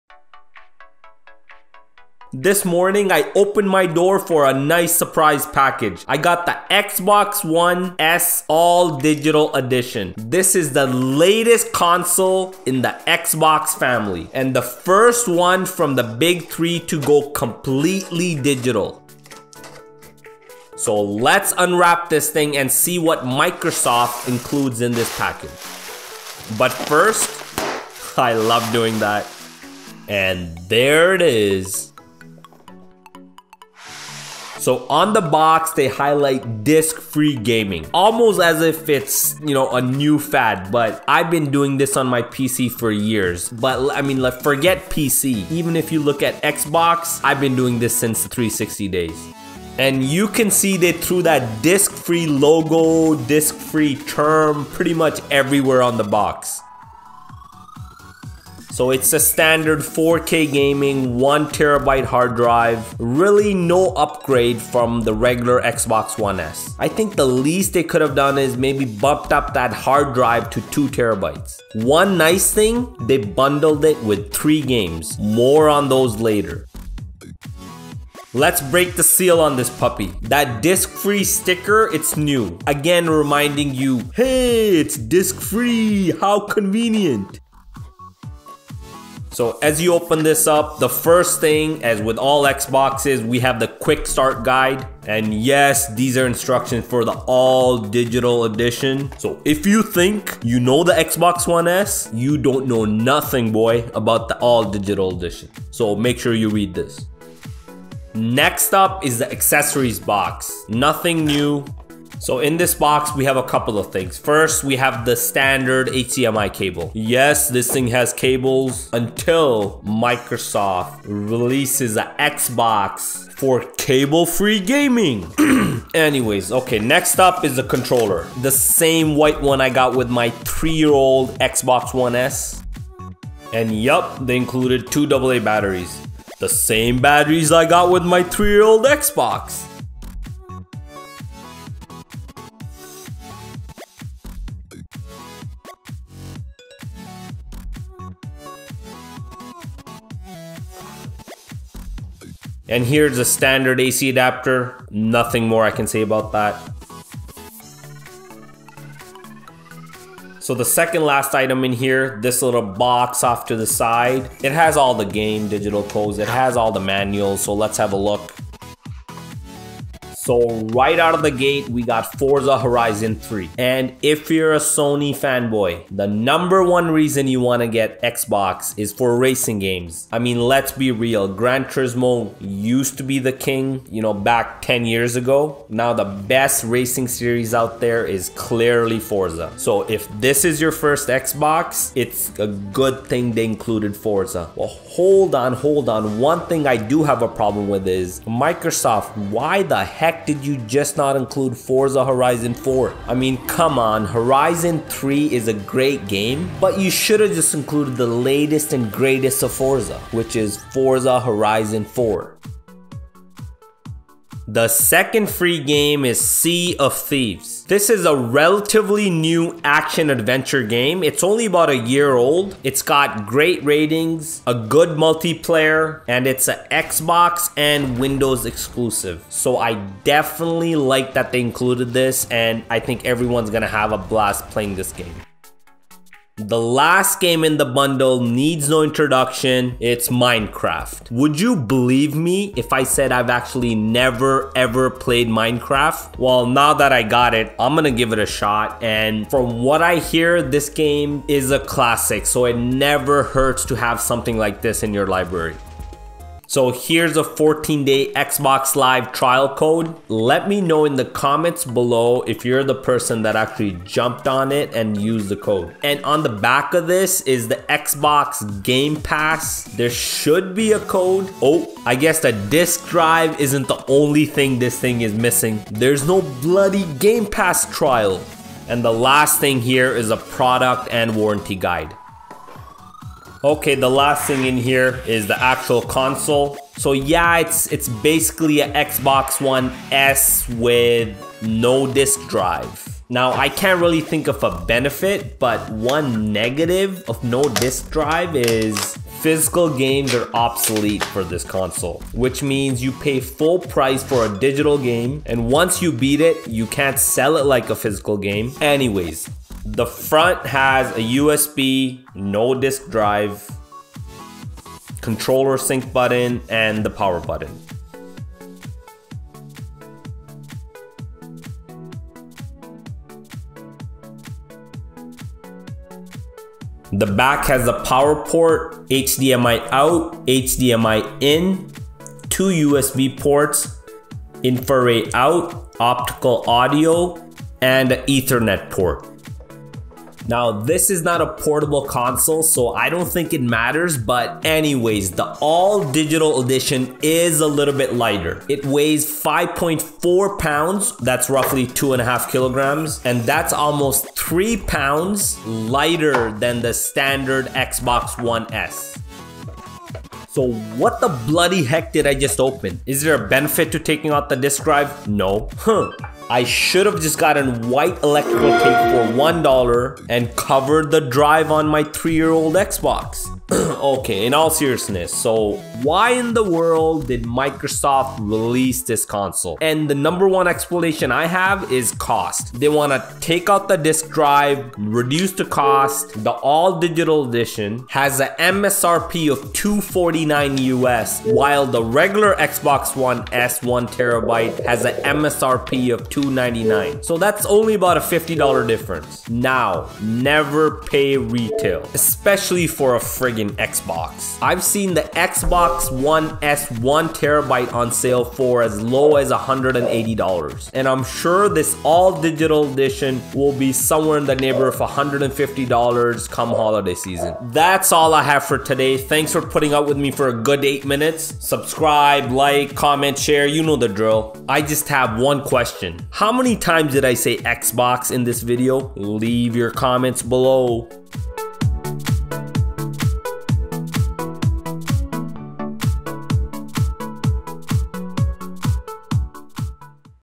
Xbox One S 1TB All-Digital Edition Unboxing and Review
BLACK FRIDAY 2019 Xbox One All Digital ON SALE FOR $150 US

This video is a review and unboxing of the Xbox one s 1TB all digital edition from Microsoft. Released on May 07, 2019 in North America.

The xbox one S all-digital edition is the latest in the microsoft xbox family of consoles. The bundle comes with includes 1TB Xbox One S All-Digital Edition Console (Disc-free Gaming), wireless controller, 1-Month Xbox Live Gold subscription, & download codes for Minecraft, Forza Horizon 3, & Sea of Thieves.

Retail price in the US is $249, in Canada its $299. This is $50 US cheaper than the Xbox one S 1TB standard bundles.